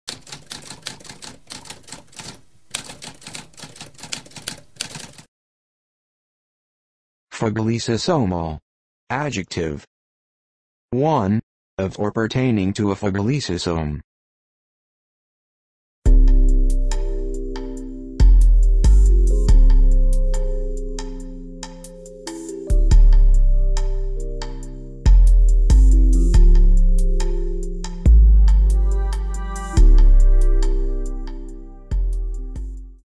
What does phagolysosomal mean?
A spoken definition of phagolysosomal.

Intro Sound:
Typewriter - Tamskp